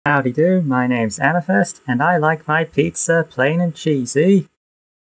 Amethyst VTuber test
I've been trying my hand at VTubing. It's a lot more difficult than I expected, especially on a screen tablet.

Art software used: Autodesk Sketchbook
VTuber Avatar created on: VRoid Studio
VTuber software used: VSeeFace
Screen recorded on: OBS Studios
Video editing software used: HitFilm Express
Audio recorded on: Audacity